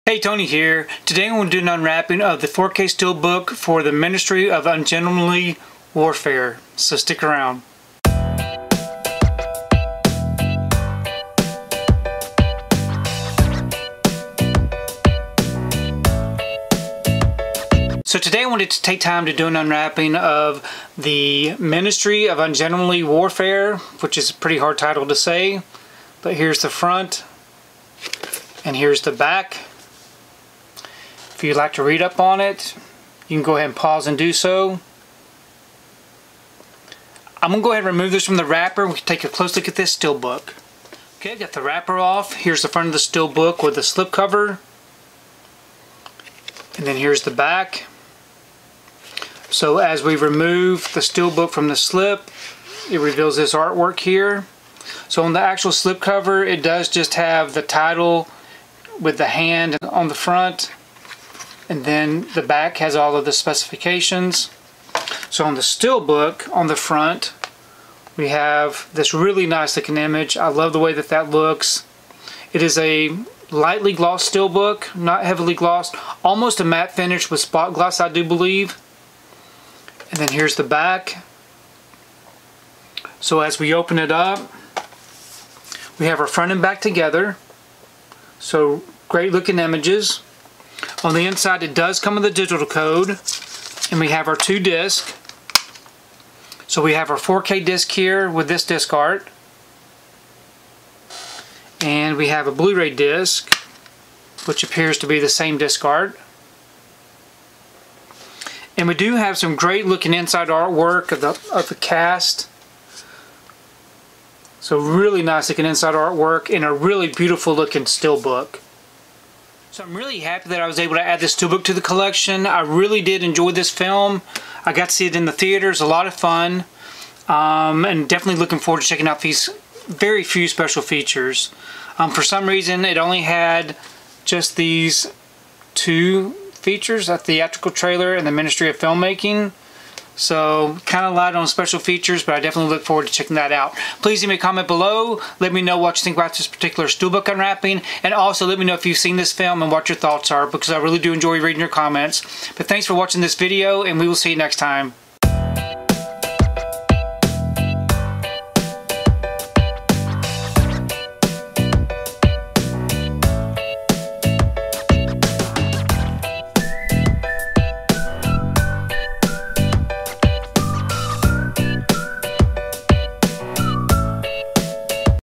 The Ministry of Ungentlemanly Warfare Amazon Exclusive 4K + Bluray + Digital Steelbook
Check out the unwrapping of The Ministry of Ungentlemanly Warfare Amazon Exclusive 4K + Bluray + Digital Steelbook!

► Purchase the 4K Steelbook
► Purchase the 4K Ultra HD
► Purchase the Blu-ray